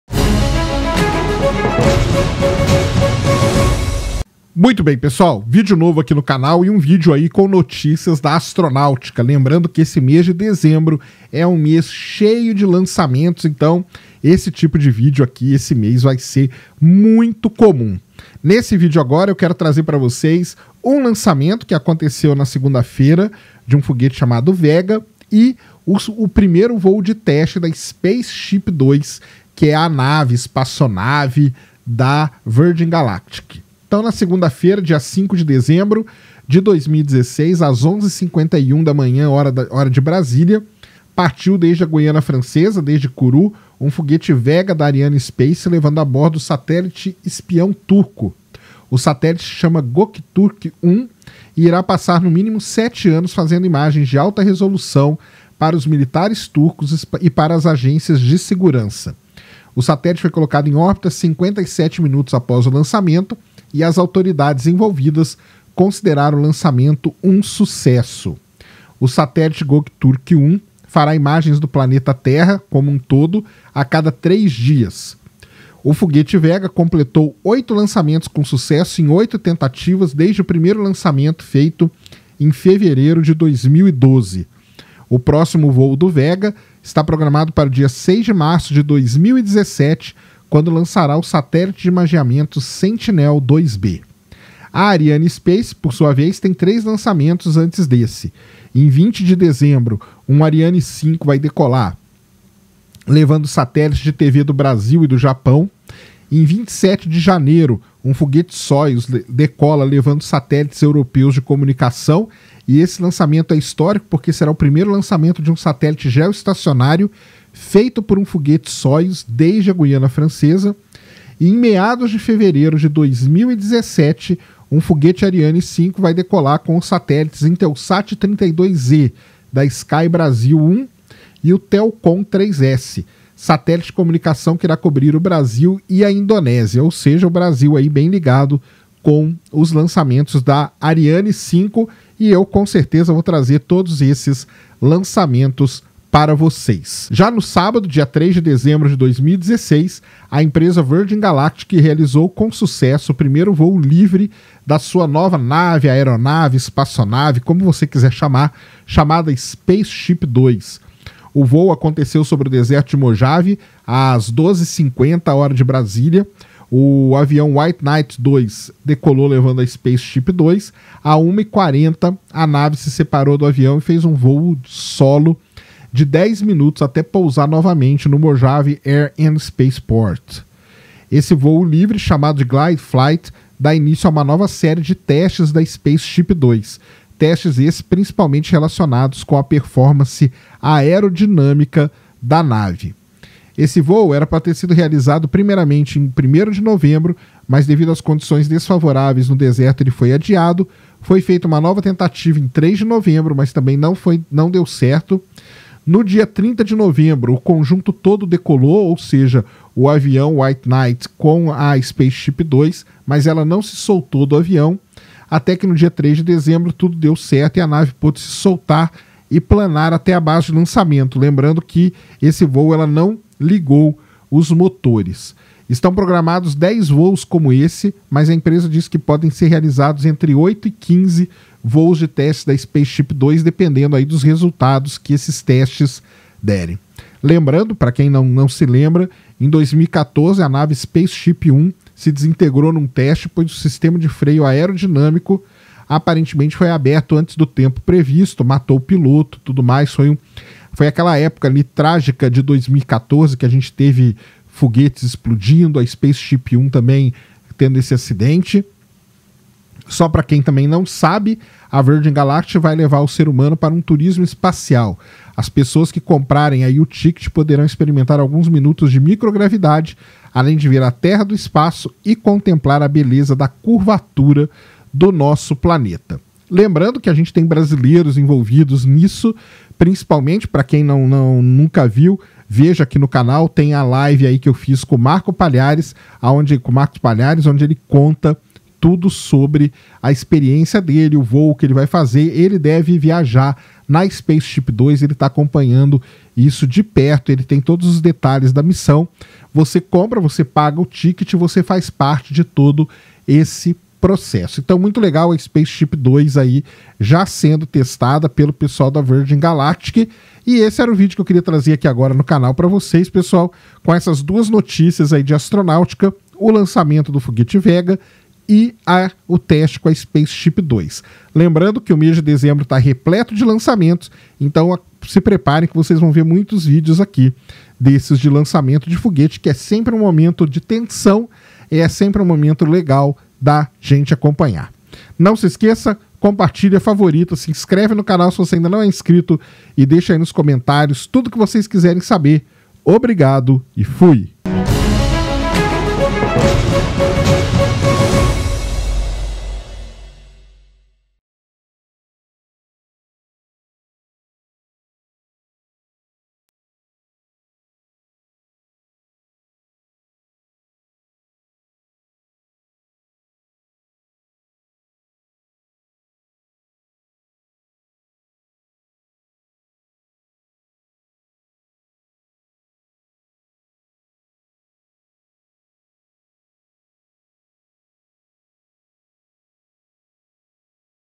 Lançamento do Foguete Vega e Teste da Spacechip 2 - Space Today TV News Ep.029
Na segunda-feira, dia 5 de Dezembro de 2016, às 11:51 da manhã, hora de Brasília, partiu, da desde a Guiana Francesa, um foguete Vega da Arianespace, levando a bordo um satélite espião turco.

O satélite se chama Gokturk 1 e irá passar no mínimo sete anos fazendo imagens de alta resolução para os militares turcos e para as agências de segurança.

O satélite foi colocado em órbita, 57 minutos após o lançamento, e as autoridades envolvidas consideraram o lançamento, um sucesso.

O satélite Gokturk 1 fará imagens do planeta Terra inteiro a cada 3 dias.

O foguete Vega, completou oito lançamentos com sucesso em oito tentativas, desde o primeiro lançamento em Fevereiro de 2012.

O próximo voo do Vega está programado para 6 de Março de 2017 quando lançará o satélite de imageamento Sentinel 2B.

A Arianespace tem 3 lançamentos antes desse.

Em 20 de Dezembro, um Ariane 5 decola levando satélites de TV do Brasil e do Japão.

Em 27 de Janeiro um foguete Soyuz decola levando satélites europeus de comunicação, e esse será o primeiro lançamento de um satélite geoestacionário feito por um foguete Soyuz desde a Guiana Francesa.

E em meados de Fevereiro de 2017, um foguete Ariane 5, decola com os satélites Intelsat 32e/sky Brasil 1  e o Telkon 3S, satélite de comunicação que irá cobrir o Brasil e a Indonésia.

No sábado, dia 3 de Dezembro de 2016, a empresa Virgin Galactic realizou com sucesso, o primeiro voo livre da sua nova nave, aeronave, espaçonave, chamada Spaceship 2.

O voo aconteceu sobre o deserto de Mojave, às 12:50, hora de Brasília, o avião White Knight 2 decolou levando a Spaceship 2, às 13:40, a nave se separou do avião e fez um voo de 10 minutos, até pousar novamente no Mojave Air and Space Port.

Esse voo livre, chamado de glide flight, dá início a uma nova série de testes da Spaceship 2. Testes esses relacionados principalmente com a performance aerodinâmica da nave.

Esse voo era para ter sido realizado primeiramente em 1 de Novembro, mas devido a condições desfavoráveis no deserto ele foi adiado, foi feita uma nova tentativa em 3 de Novembro, mas também o teste não foi realizado.

No dia 30 de novembro o conjunto decolou novamente, mas dessa vez a Spaceship 2 não se soltou do avião. Até que no dia 3 de Dezembro tudo correu bem e a nave pôde se soltar a planar até a base de lançamento.

Estão programados 10 voos como esse, mas a empresa disse que podem ser realizados entre 8 e 15 voos de testes para a Spaceship 2.

Em 2014, a nave Spaceship 1 se desintegrou num teste, pois o sistema de freio aerodinâmico aparentemente foi aberto antes do tempo previsto.

A Virgin Galactic irá levar o ser humano para um turismo espacial. As pessoas que compraram o ticket poderão experimentar alguns minutos de microgravidade, além de ver a Terra do espaço e contemplar a beleza da curvatura do nosso planeta.

Lembrando que temos brasileiros para embarcar nesse voo, um deles é o Marco Palhares que fez um hangout com a gente no canal e explicou todos os detalhes dessa missão.

Conheça a Agência Marcos Pontes e torne o seu sonho de conhecer o ESO em realidade:

A nova casa do SciCast

Obrigado pela audiência e boa diversão!!!